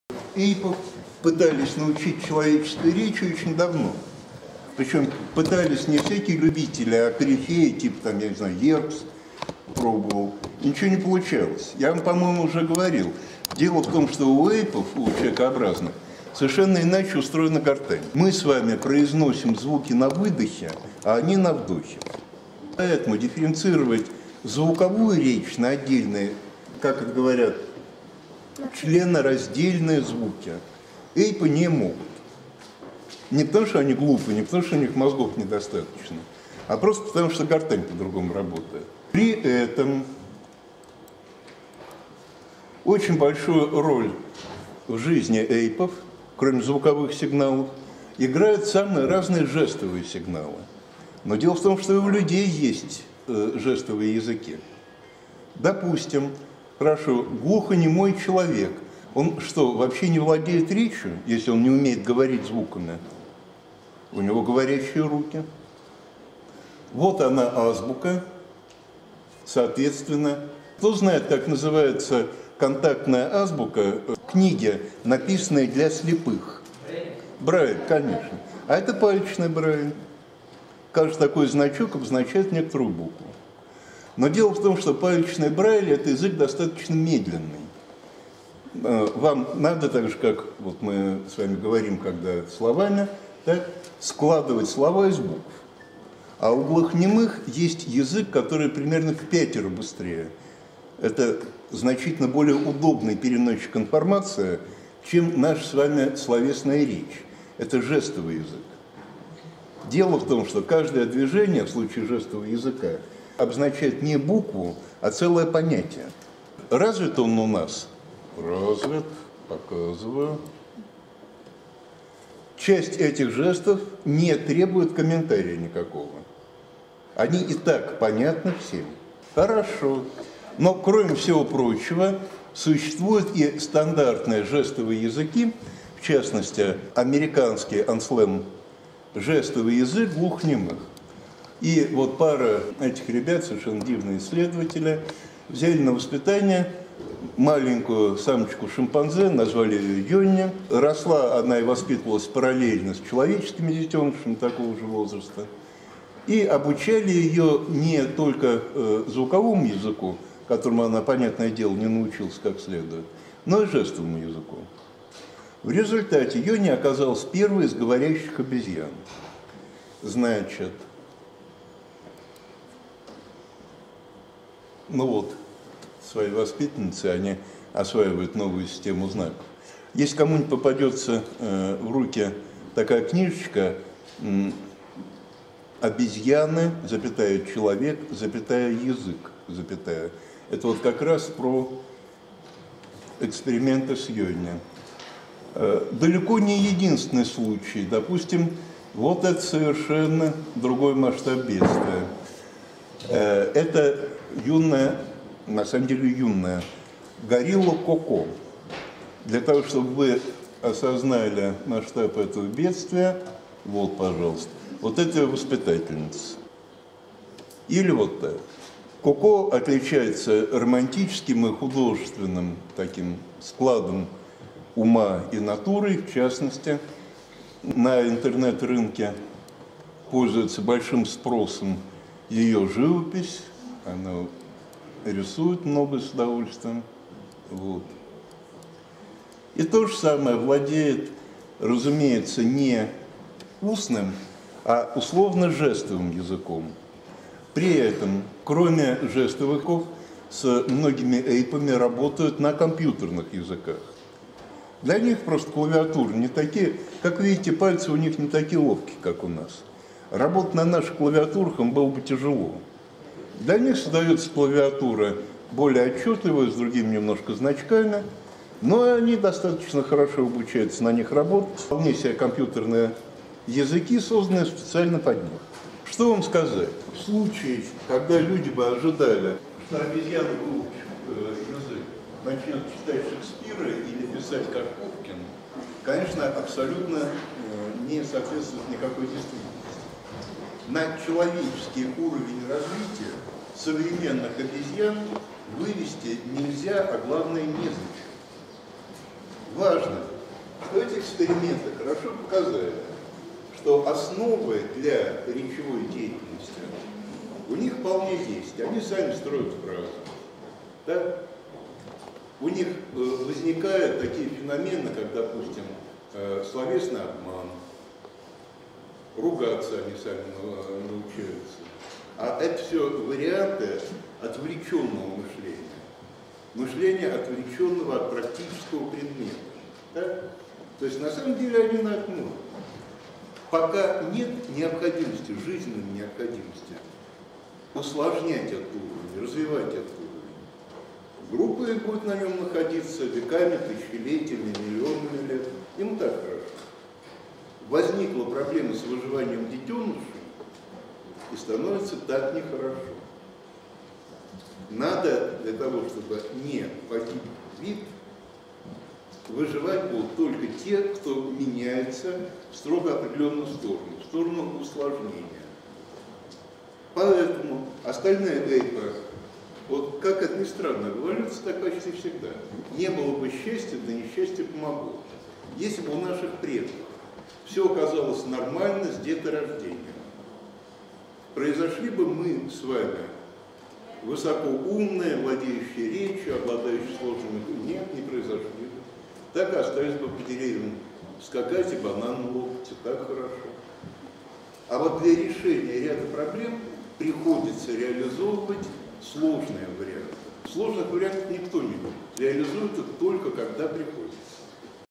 20 Коко, Юни и других обезьян учим разговаривать
Лекция Андрея Николаевича Квашенко на Малом мехмате МГУ 19 марта 2016 года.